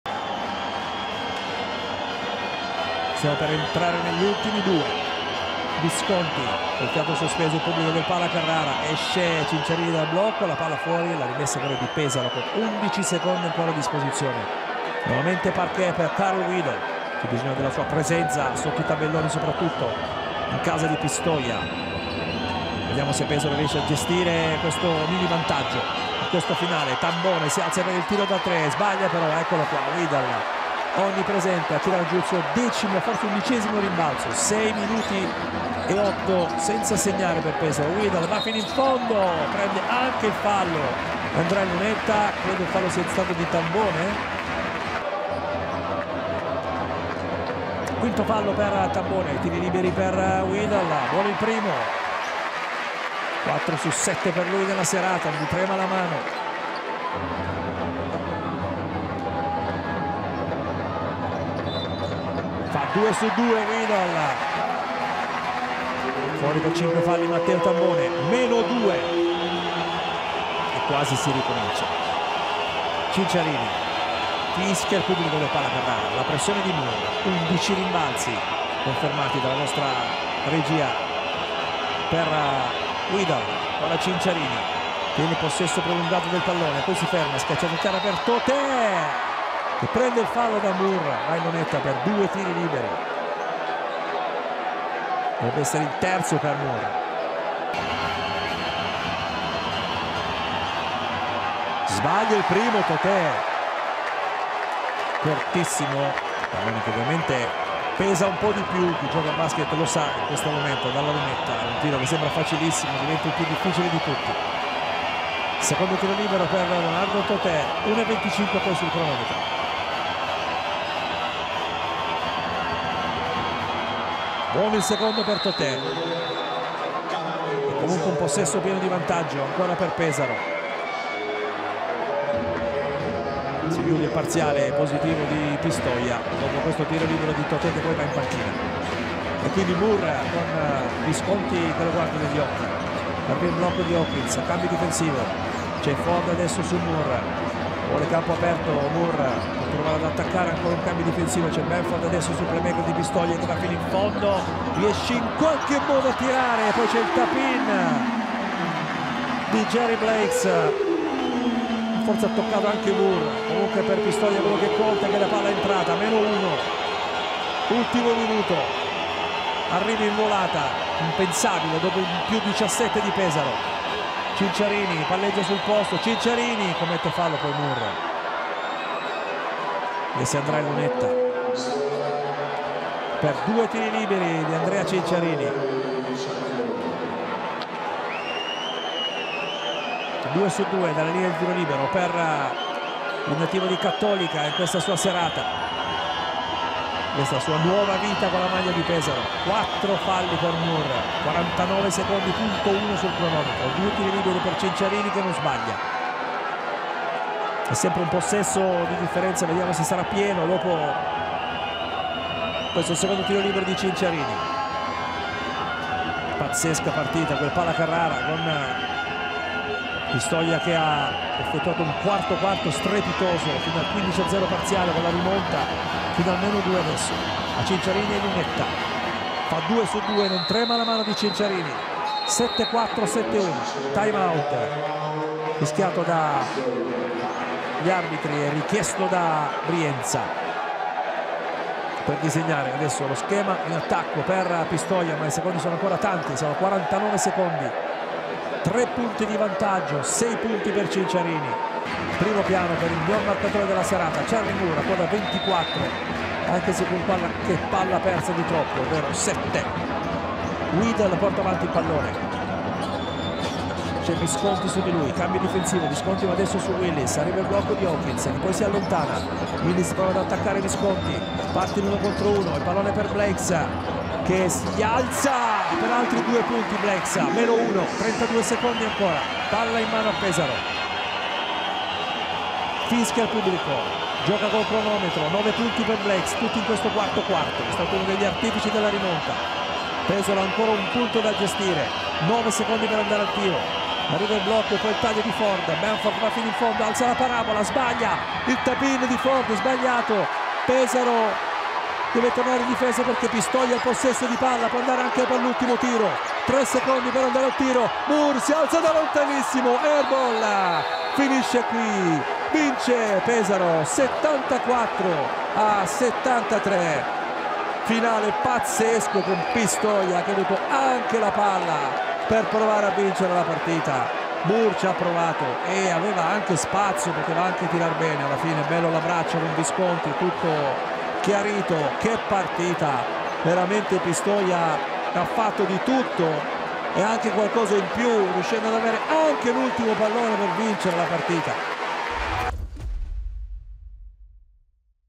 🚨 Sulla sirena: l'incredibile finale di Pistoia 🆚 Pesaro
I due liberi di Cinciarini rimettono un possesso pieno di distanza ma Blakes viene pescato da sotto per il 73-74. Bamforth fallisce il colpo del k.o. e Moore ha tra le mani la palla della vittoria ma il suo tentativo da tre punti non trova neanche il ferro! Vince Pesaro! 🏀
Riviviamo le ultime azioni della partita! 😮